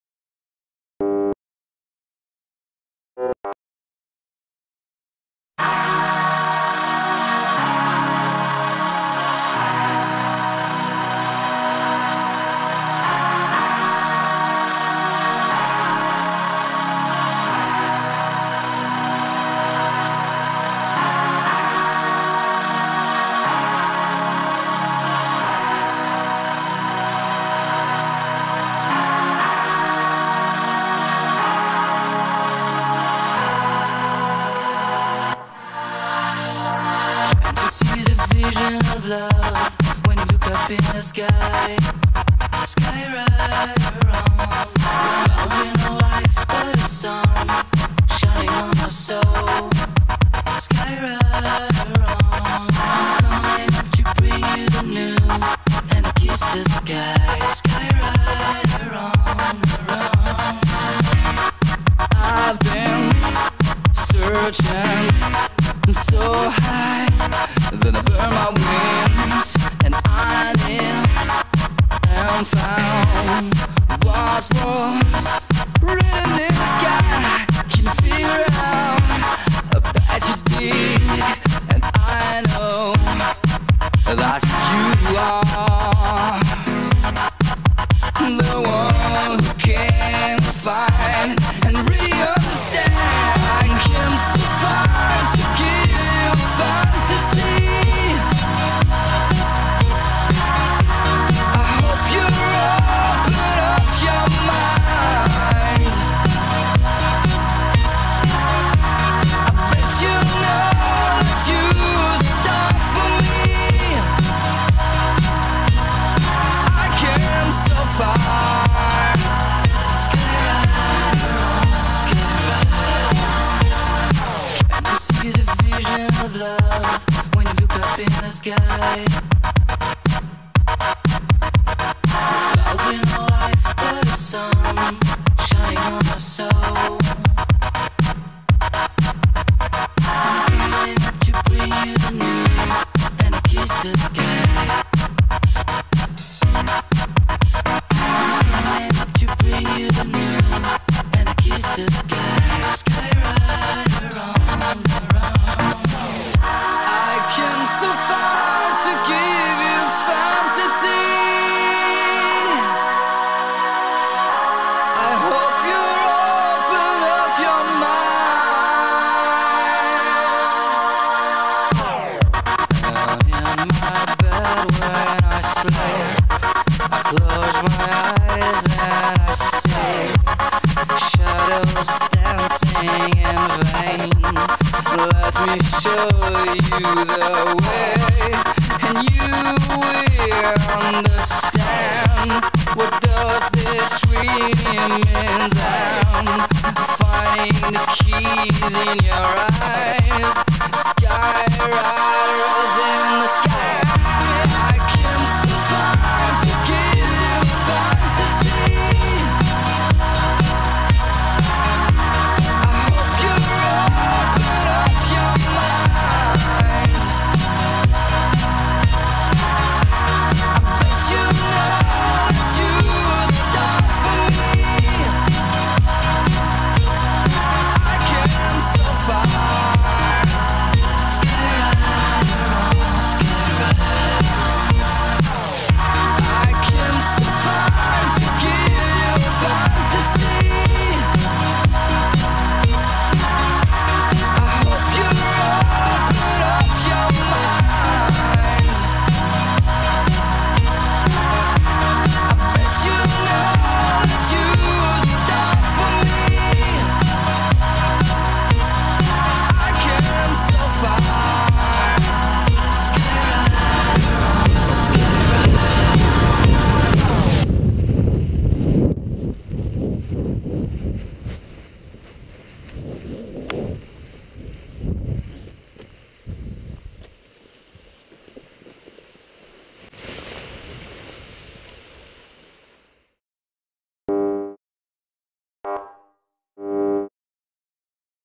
Vol rando refuge de Larrieux - Crêt des Tervelles
Vol randonnée au départ du lieu dit : Les Pesets, vallée de Cotagne au Sud Ouest de Thônes.
1000 mètres de dénivelé vers le Crêt des Tervelles en passant par  le refuge de Larrieux. Décollage au Nord Est du Crêt, 30 mètres en contre bas ( 1750 mètres environ )
Descente en DUDEK Zakospeed 16V airlight et U TURN Bodyguard pour Julien